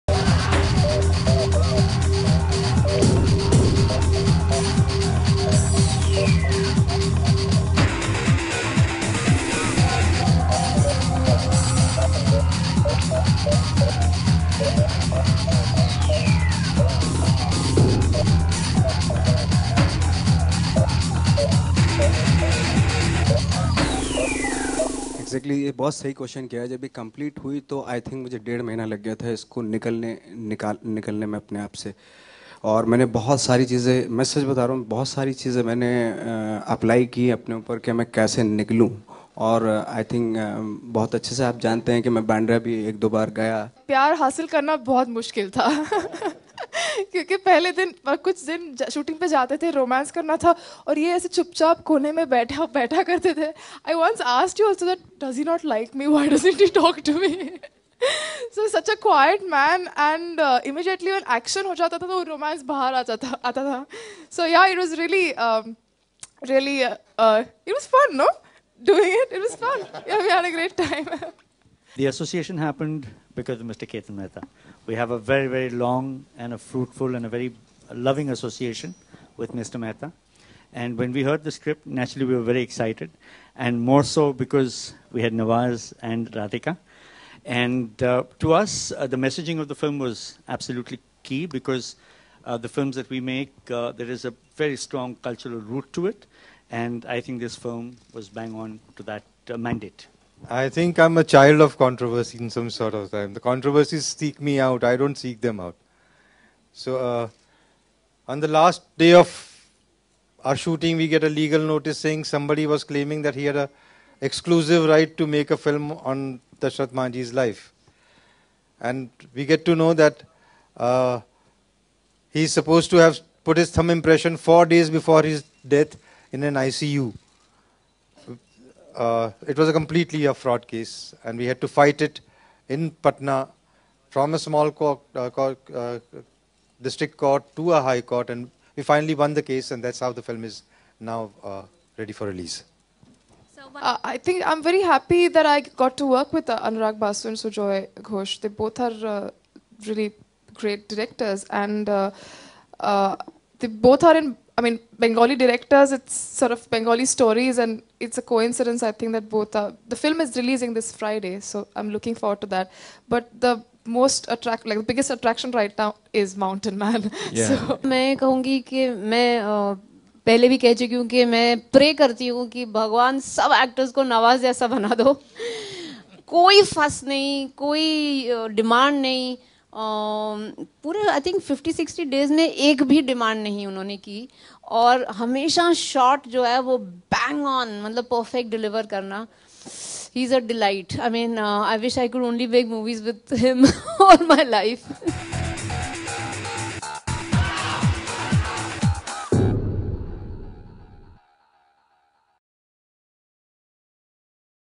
Radhika Apte and Nawazuddin Siddiqui at "Manjhi" Trailer Launch | New Bollywood Movies News 2015
Radhika Apte and Nawazuddin Siddiqui at "Manjhi" Trailer Launch
Manjhi is an upcoming Indian biopic based on the life of Dashrath Manjhi, popularly known as the ‘Mountain man’, who lived in Gehlaur village, near Gaya, in Bihar. It is directed by Ketan Mehta and produced by Viacom18 Motion Pictures, Maya Movies Pvt. Ltd. & NFDC. It features Nawazuddin Siddiqui and Radhika Apte in the lead roles. The film is scheduled for release on 21 August 2015

The movie is based on the life of Dashrath Manjhi (Nawazuddin Siddique) who carved a road through a mountain in 22 years

Nawazuddin Siddiqui as Dashrath Manjhi
Radhika Apte as Phaguniya-Dashrath’s wife
Pankaj Tripathi Ruab the landlord’s son
Gaurav Dwivedi as Alok Jha a journalist
Ashraful Haq as Magru- Dashrath's father
Jagat Rawat as Shuklaji
varadraj swami as gopal father

The music in the movie has been composed by Sandesh Shandilya, with lyrics by Ketan Mehta and Deepak Ramola.